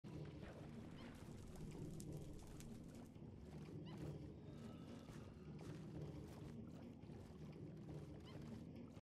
Underwater bedbugs 1.19.81
mobs stuck spinning underwater